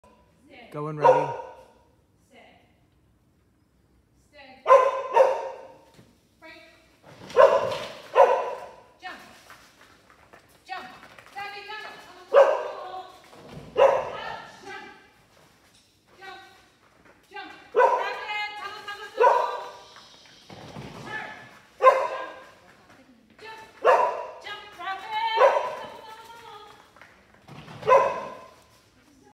Savvy 23 Sep
Cindy and Savvy run IDAL's September '23 game "Puppy Cannon", 4 tunnels + 3 jump sequences.